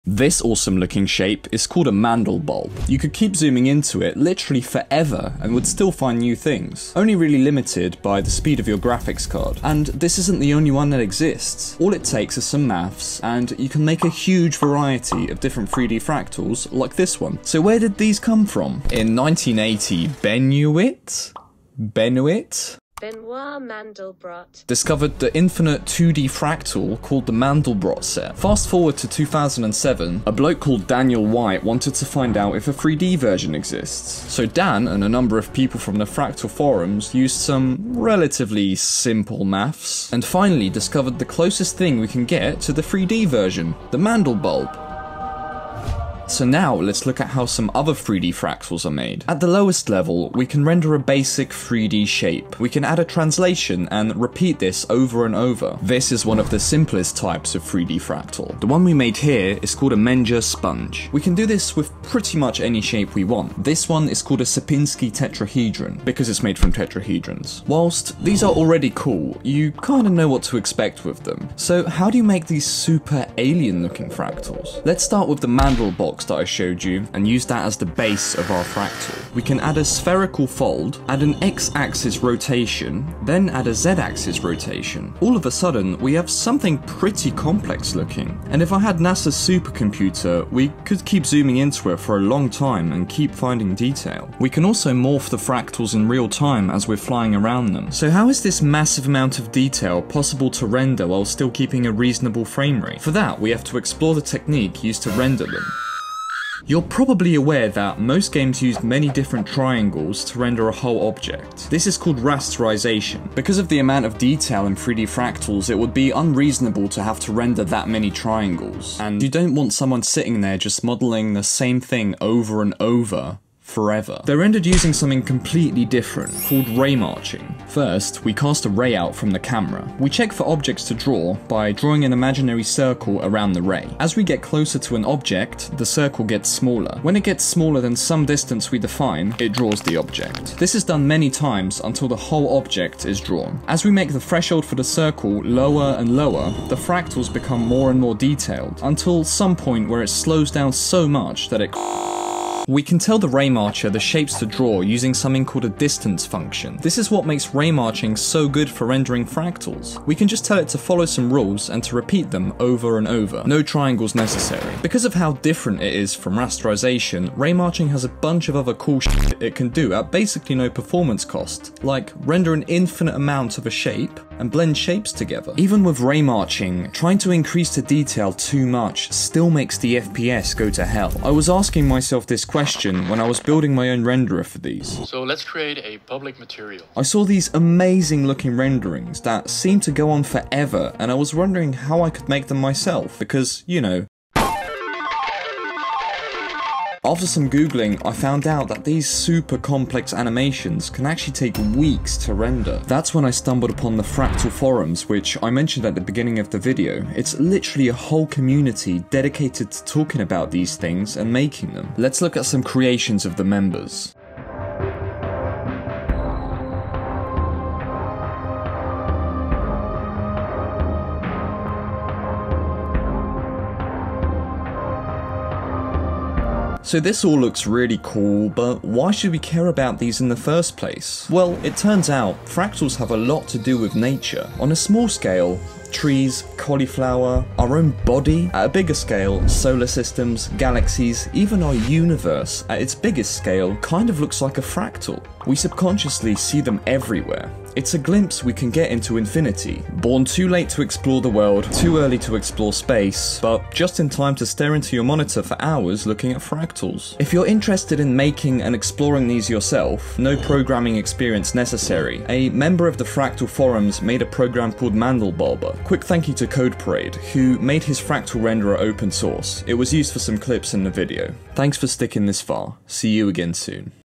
I Made 3D FRACTALS In Unity!
This video takes you into the mysterious world of 3D Fractals and the Mandelbulb. I show you how they are made and show you around a few. I make a renderer in the Unity Game Engine which uses Ray Marching, a really cool rendering technique.

0:00 The Mandelbulb
0:45 From Basic Shapes To 3D Fractals
1:36 Ray Marching
2:55 Fractal Forums And Super Detailed Renderings
3:46 Why Should We Care?